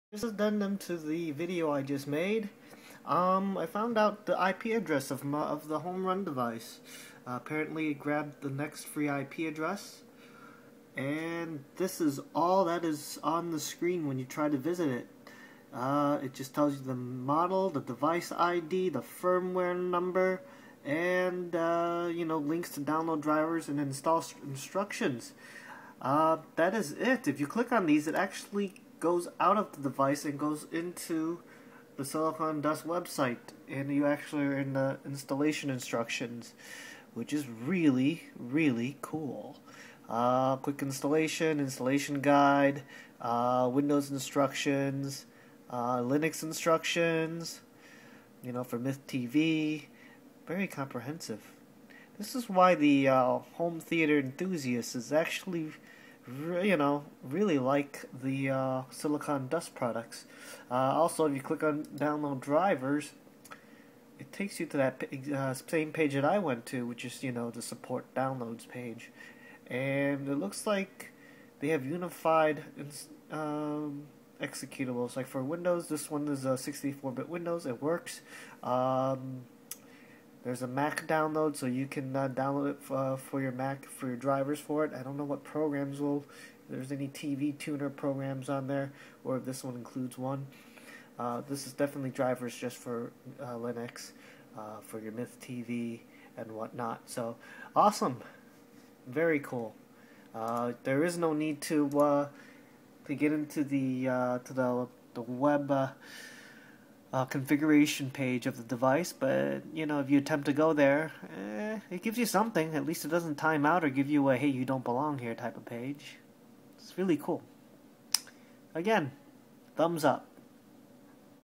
GuyTechie HDHomeRun Web Interface
An addendum to my SiliconDust HDHomeRun video review.  I stated earlier that this thing was very easy to install that you don't even need to configure any network settings on it as long as you have a DHCP server in the house (your typical router that allows sharing of your internet connection would have one).

Anyways, I found the IP address by logging into my router and look at the DHCP table to see what devices are using which IPs.  After I found the IP address the HDHomeRun uses, I attempted to browse to it using a web browser.  There's something there!  But there's not much.

 Of course, now I wonder how this thing works if your network uses static IPs.